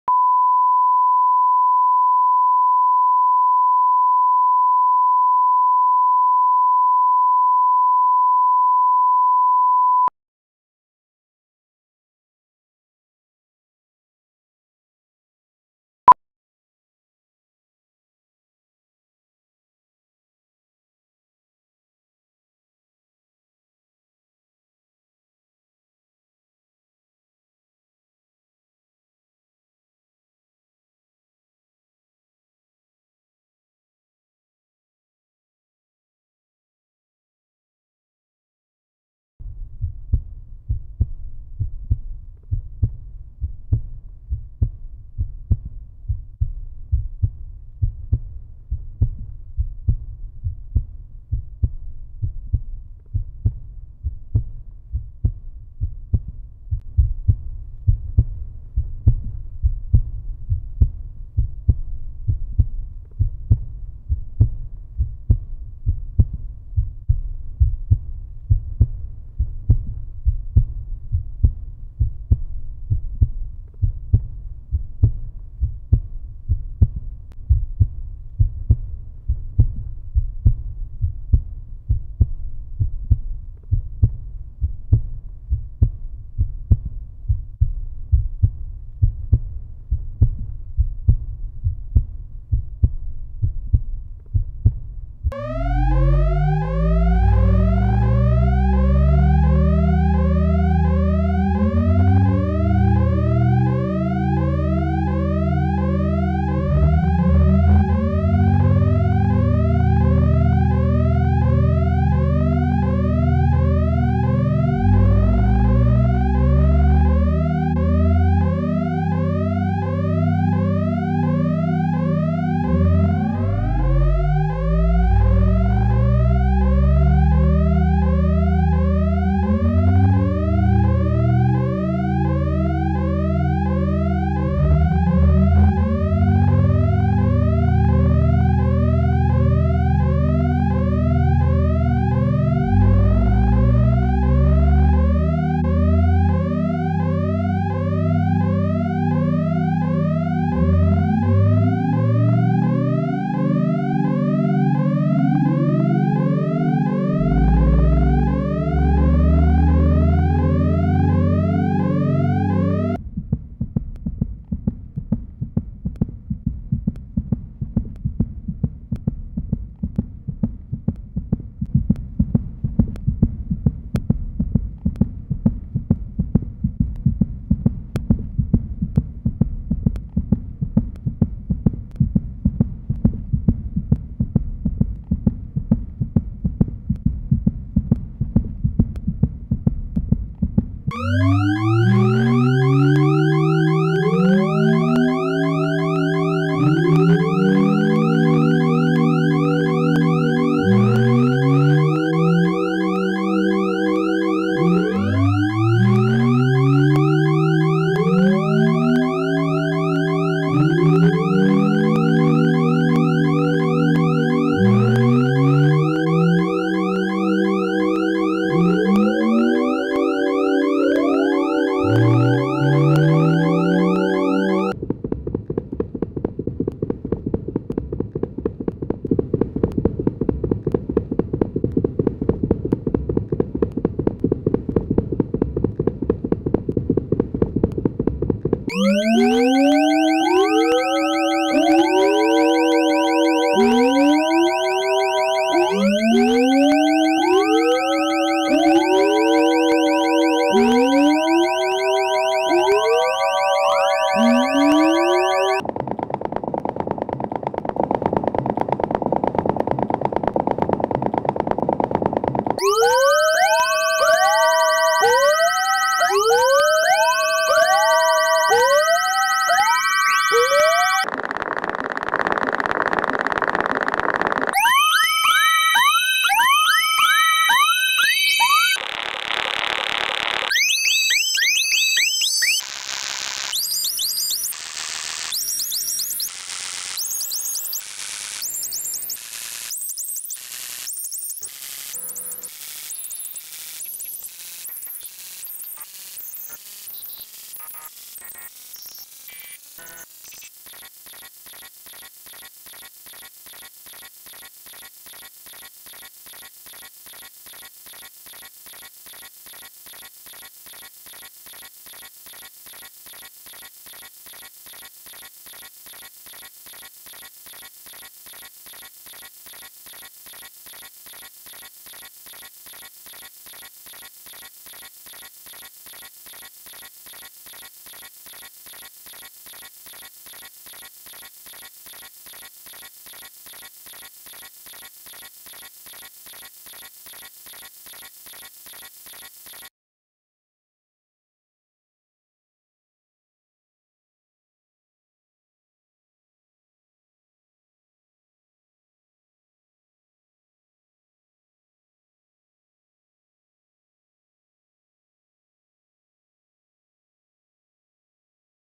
"The Walk" Experimental Film
Experimental film inspired by Tony Conrad's "The Flicker" from 1965